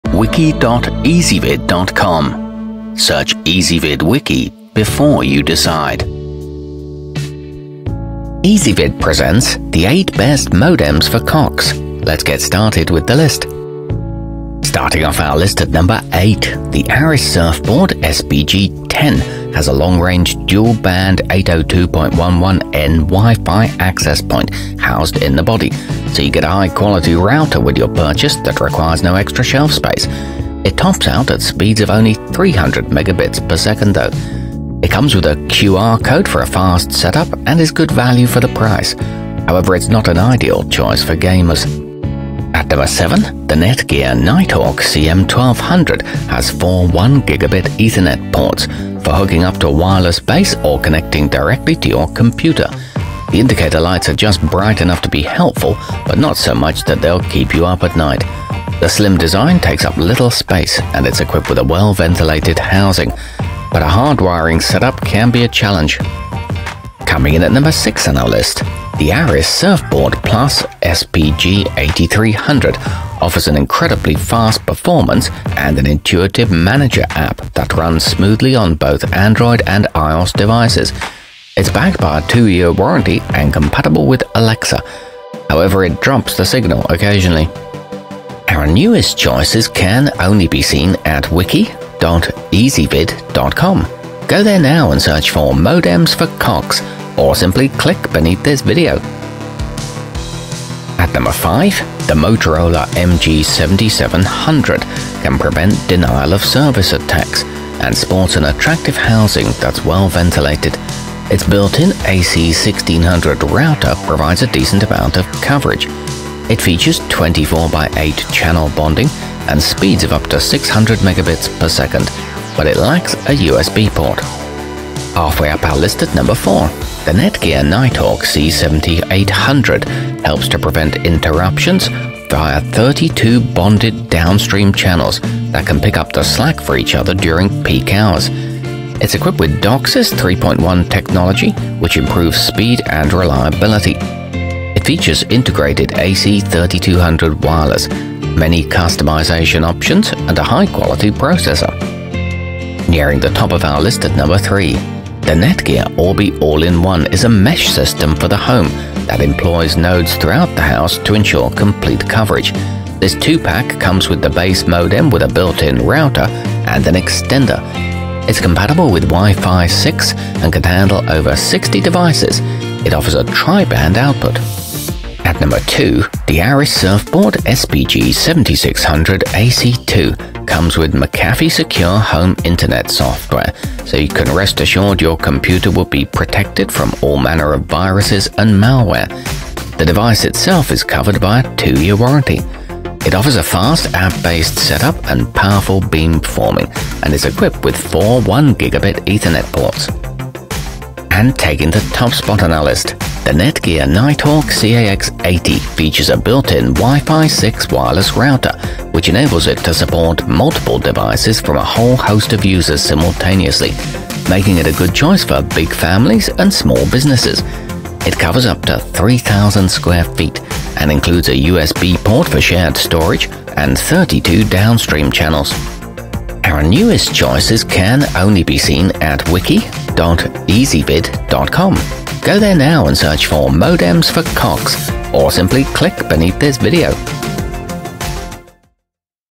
8 Best Modems For COX 2020
Our complete review, including our selection for the year's best modem for cox, is exclusively available on Ezvid Wiki.

Modems for cox included in this wiki include the netgear nighthawk cax80, arris surfboard sbg7600 ac2, arris surfboard plus sbg8300, netgear nighthawk cm1200, arris surfboard sbg10, motorola mg7700, netgear orbi all-in-one, and netgear nighthawk c7800.